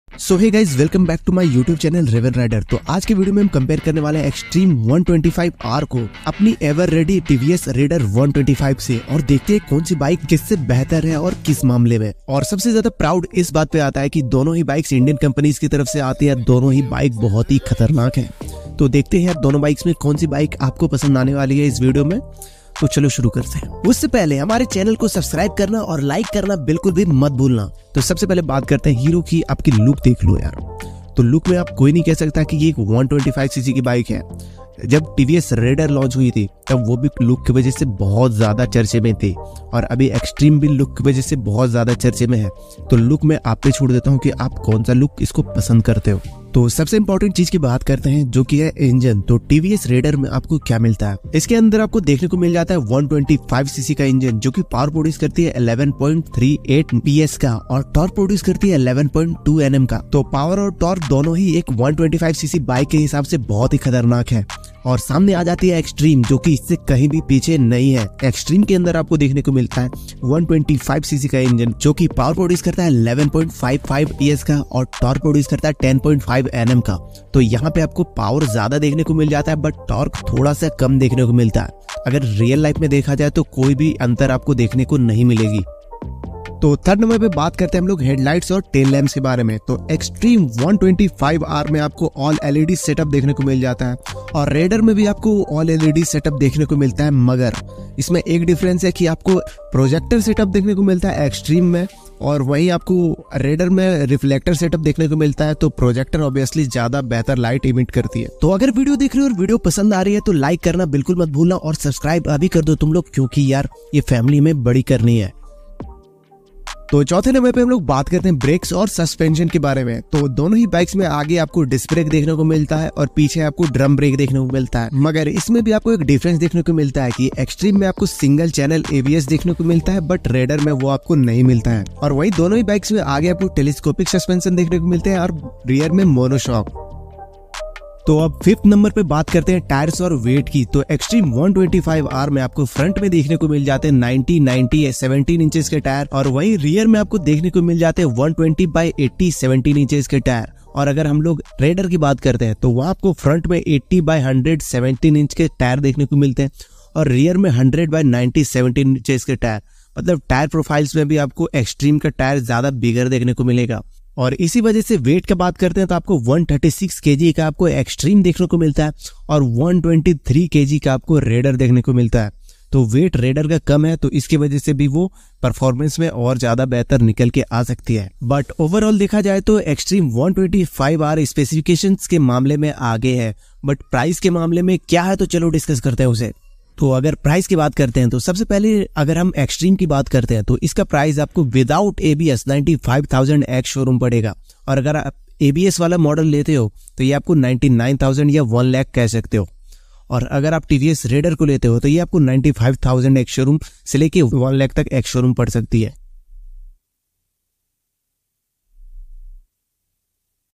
2024 Hero Xtreme 125R VS TVS Raider 125 Full Comparison | Which One Should You Buy 😍😍
2024 Hero Xtreme 125R VS TVS Raider 125 Full Comparison | Which One Should You Buy 😍😍

Raven Rider Ranchi

Kindly

00:00 Introduction
00:28 Look
00:47 Engine
01:26 Light setup
01:54 Brakes
02:11 Suspension
02:17 Tyres
02:44 Weight
03:04 Price